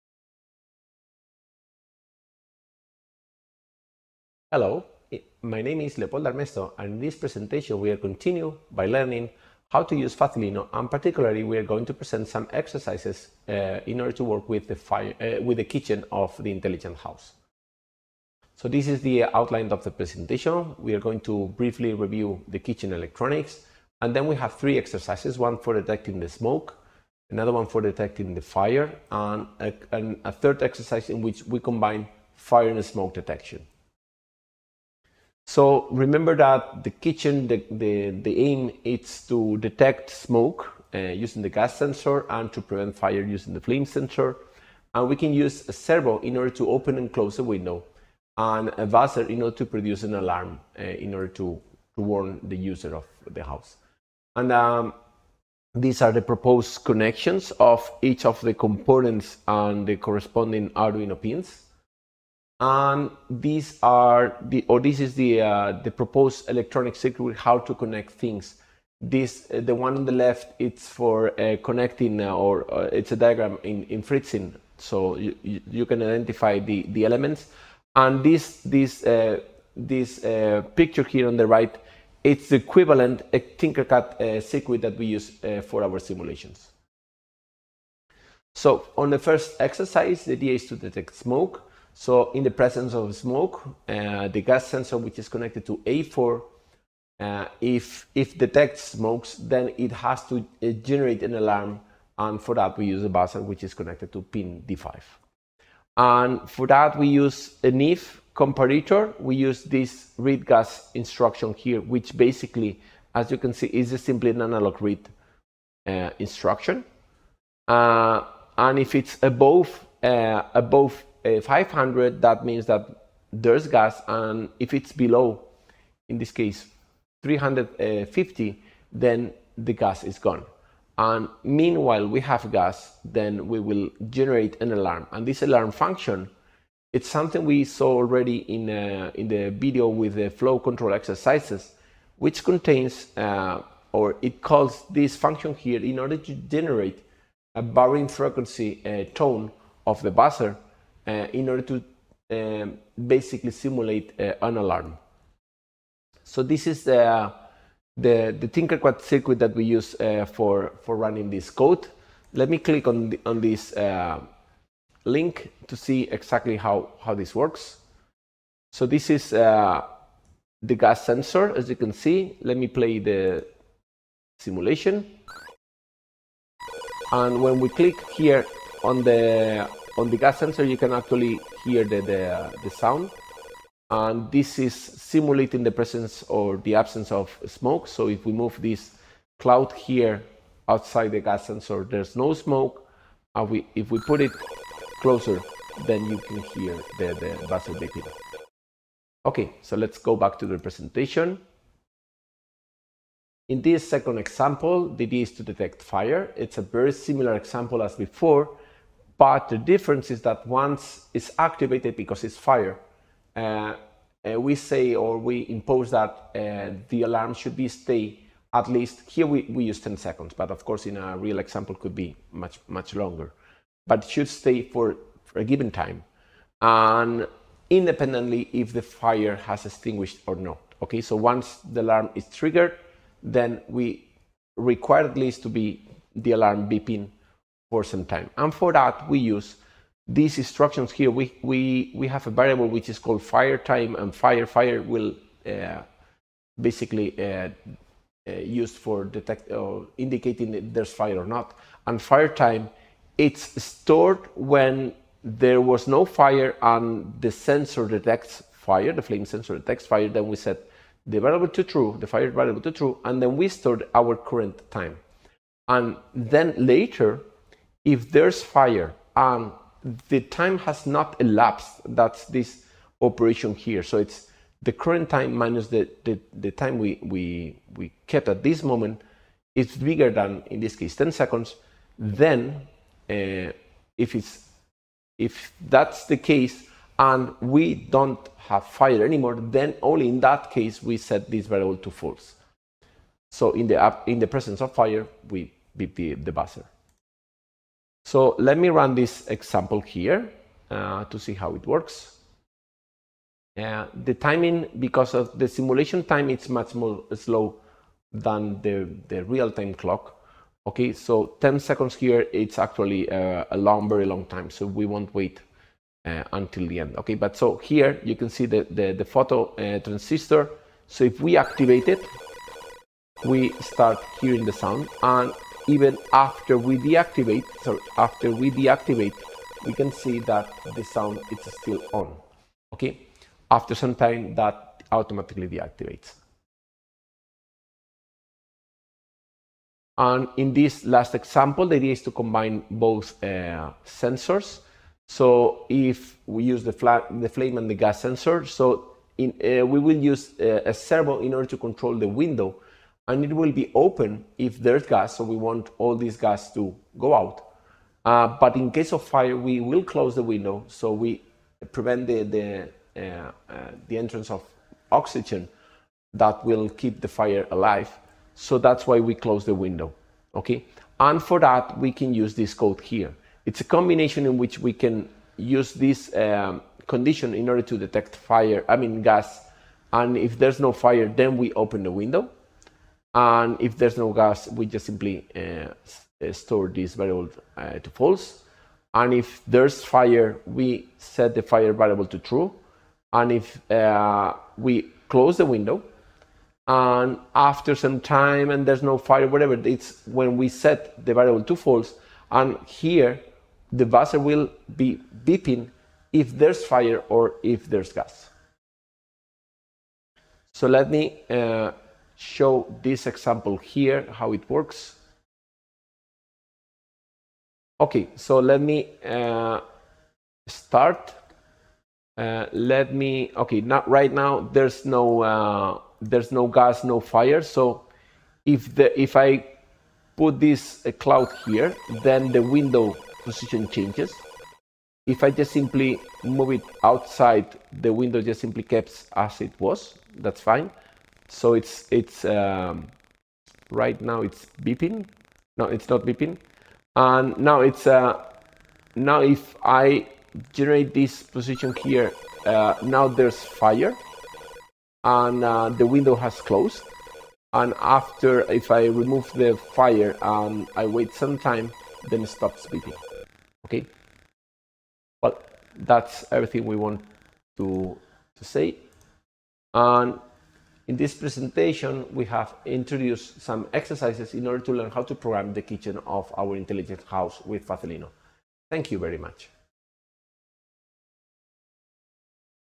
EcoThings: Programming. Kitchen Smoke Detection
This video shows some basic exercises to start programming smoke detection and fire prevention for the kitchen of an intelligent house with Facilino.

This video is part of a set of videos in a MOOC within the EcoThings Project (2021-1-ES01-KA220-SCH-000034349), funded by SEPIE within the ERASMUS+ programme.